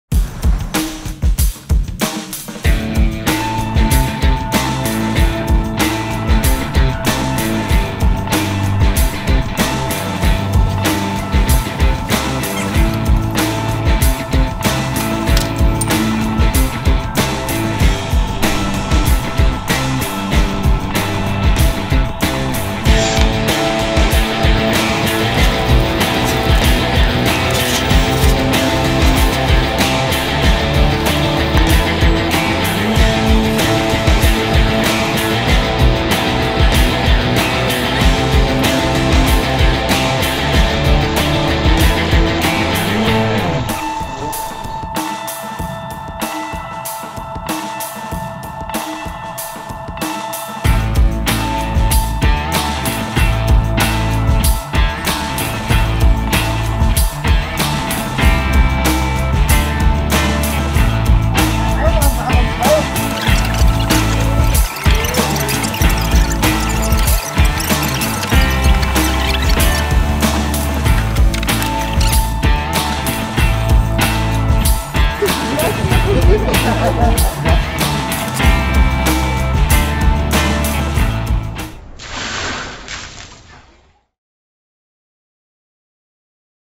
Welches Bier löscht am besten? Der ultimative Test
Normale Flasche, Dose, PET-Flasche oder Bügelflasche? Mit welcher Art Bier löscht man am besten ein Feuer? Anlässlich einer wahren Begebenheit - Zeit für den ultimativen Test!